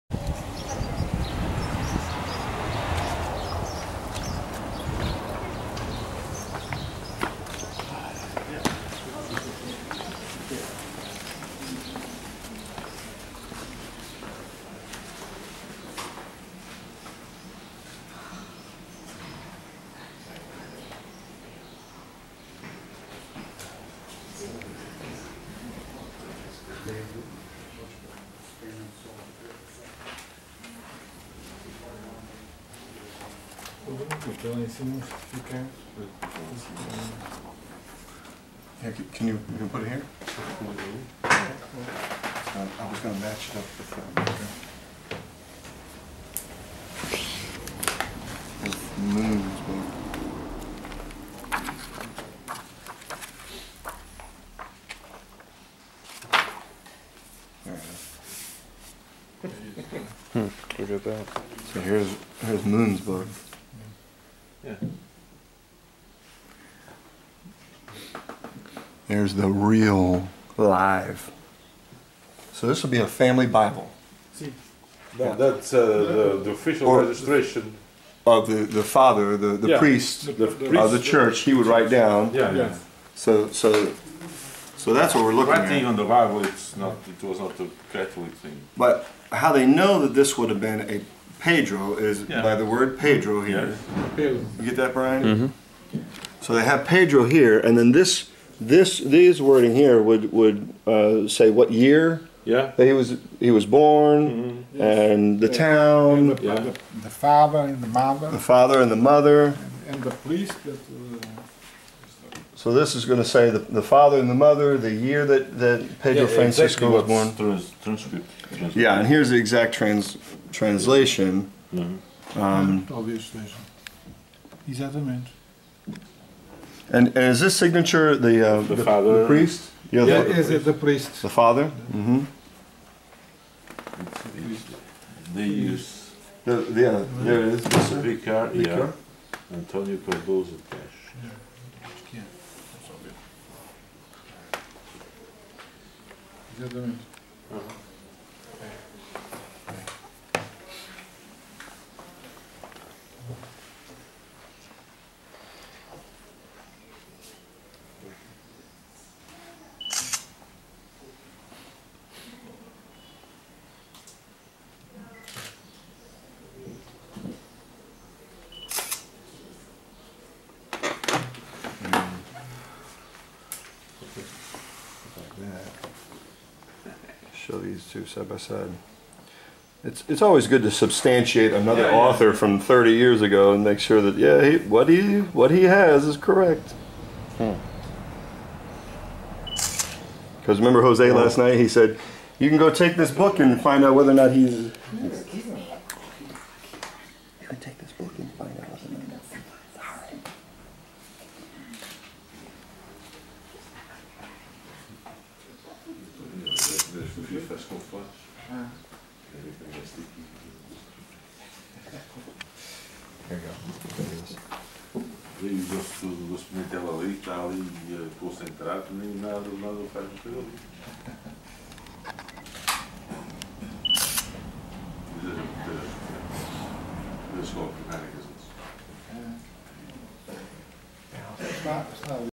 Peter Francisco's Birth Record
I visited Angra's Archives & Library which holds the birth record for Peter Francisco.  The Director of the Library gave me a certificate that verifies that this is the very birth record of the Hercules of the Revolution.